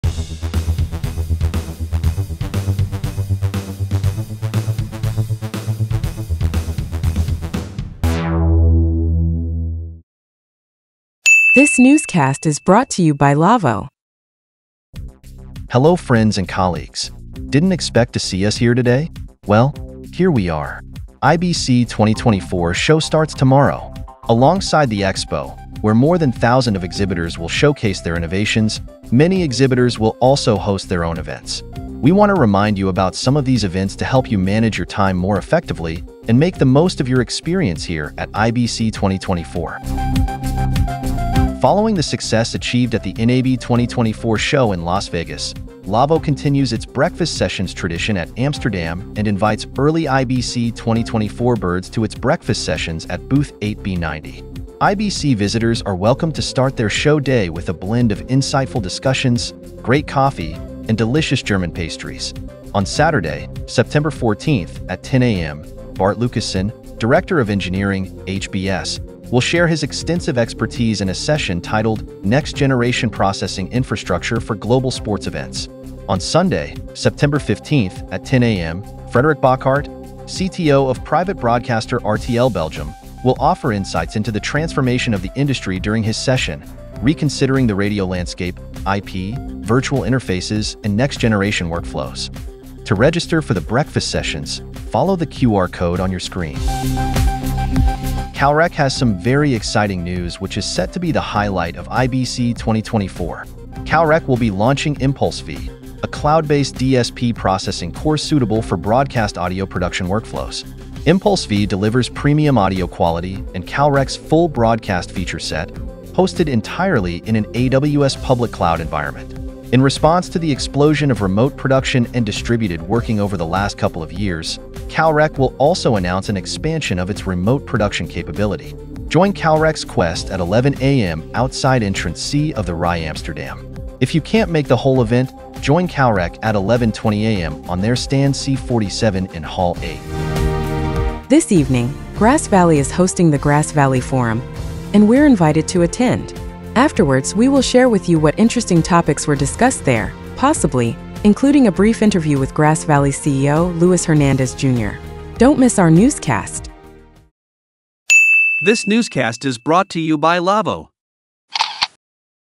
Briefly 178 by MediaVision News
Hello friends and colleagues. We welcome you to Amsterdam. The IBC2024 Show starts tomorrow, and we want to remind you about a few exciting events you're welcome to attend. Don't miss them , as well as our newscasts from the show floor and the events happening alongside the show.